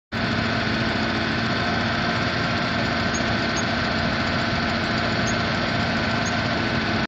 1994 Ford F250 XL pickup truck for sale at auction | bidding closes June 4, 2019
1994 Ford F250 XL pickup truck
274,774 miles on odometer
5.8L V8 OHV 16V gas engine
Automatic transmission
AC and heat
235/85R16 tires
Cracked windshield
Weak battery
Driver's side door handle inoperable
Unknown operating condition
Unit# 108
Kansas title
1FTEF25HXRLA58806
It's located in Girard, KS, and we're selling it to the highest bidder regardless of price.